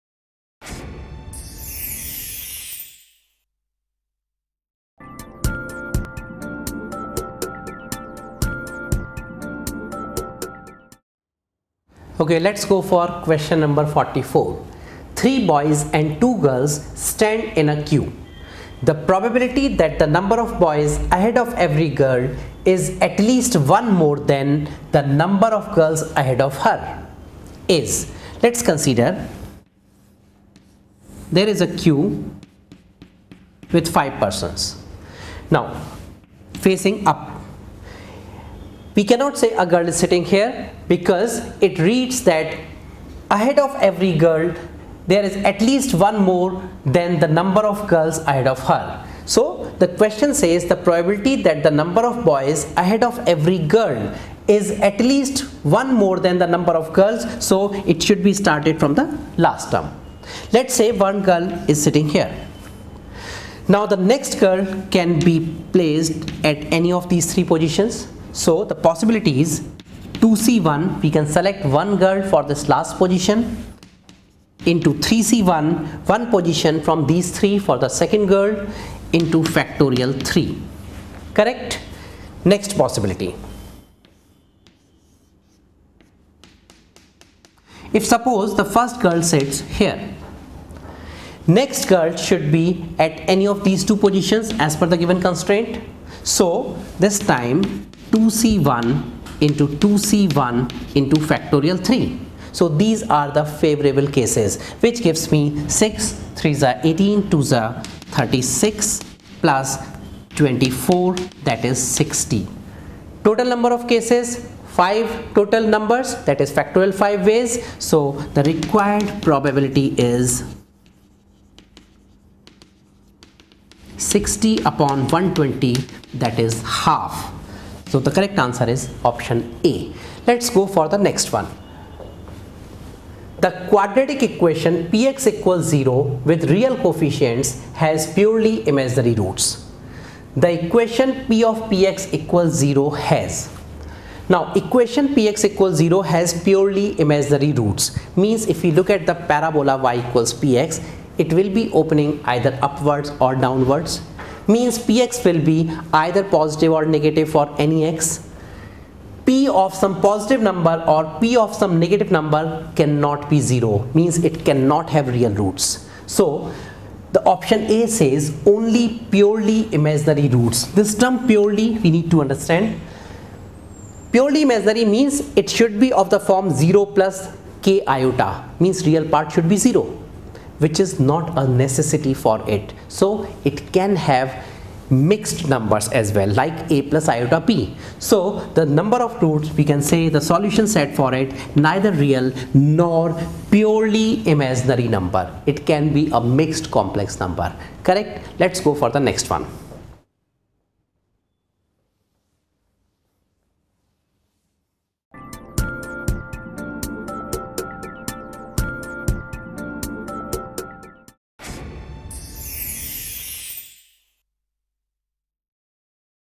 JEE (Advanced) 2014 Solutions-Maths Paper-2 [Q-44 to Q-45] By Aakash
Find JEE (Advanced) 2014 Video Solutions of Mathematics Paper-2 from Question No. 44-to-45 (Code-0) by Aakash Institute. [Apply here for JEE (Advanced) 2015 Droppers-Course